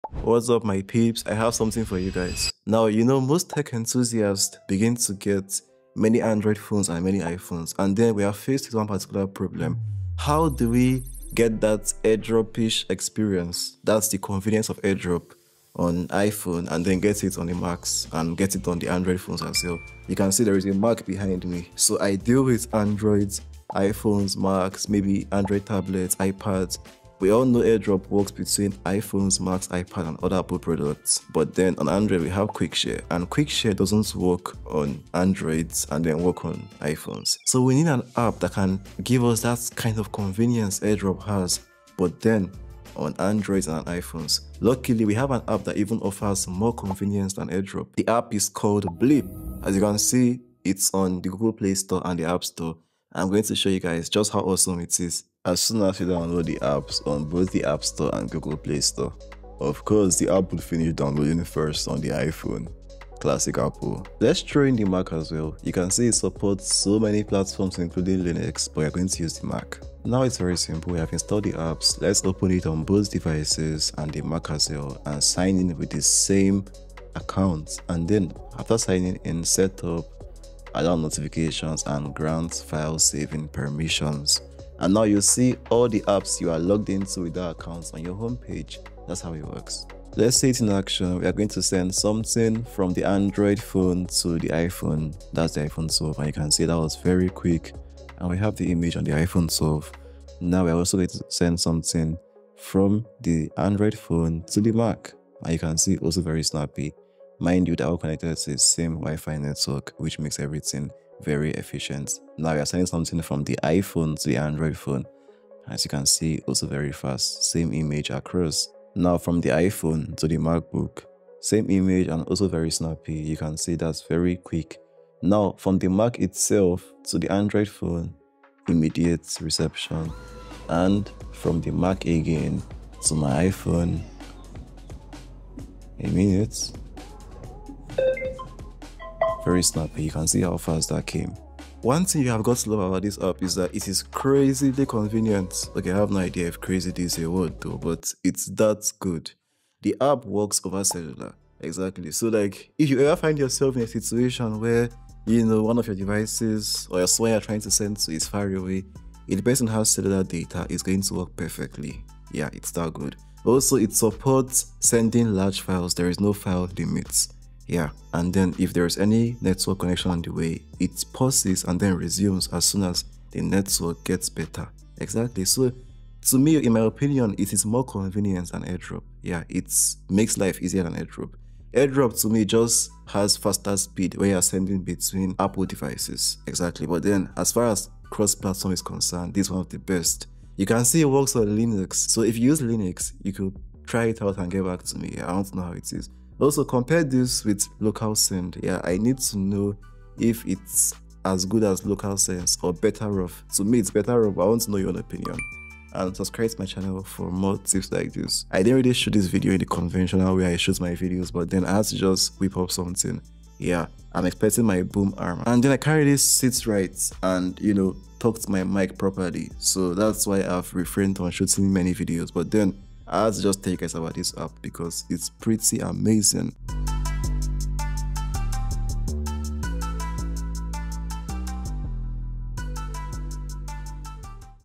AirDrop from iOS to Android and Linux is FINALLY Possible
An app that can let you Airdrop files between iOS, android, mac, linux, and windows devices. sounds too good to be true but it isn't.

0:00 intro
0:51 downloading it
1:11 setup
1:29 testing it out
2:21 recommendations